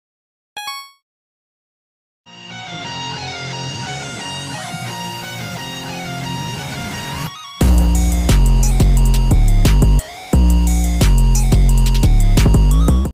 Risk.. || Gacha Trend || Warrior Hazel AU || ! Read desc for oc info ! || OG? || Gc
Characters:
Black red hair girl - Fatimah
Girl with the hijab - Hazel
Boy with purple eyes - Zafran
Boy with one eye covered - Shafiq

Reason: studying for a REALLY important exam.